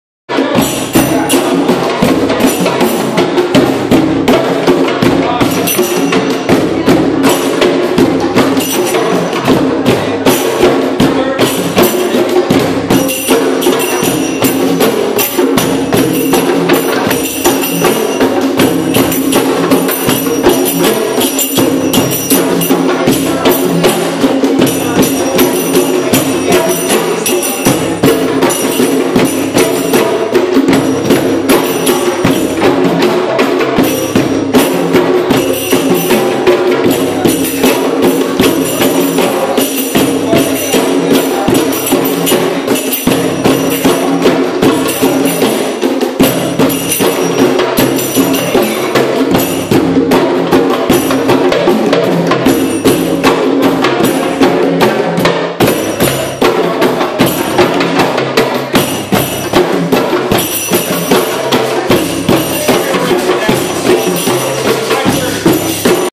La Villa Full Moon Drum Jam in Boquete Panama
A great night at Cafe La Villa with indoor fire and some belly dancing.